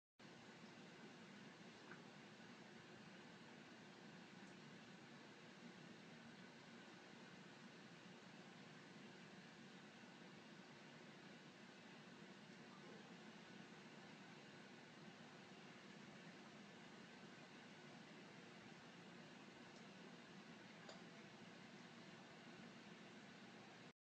Biker Faceplants Into Truck
That's what you get for wearing a pink jumpsuit on a motorcycle in the rain.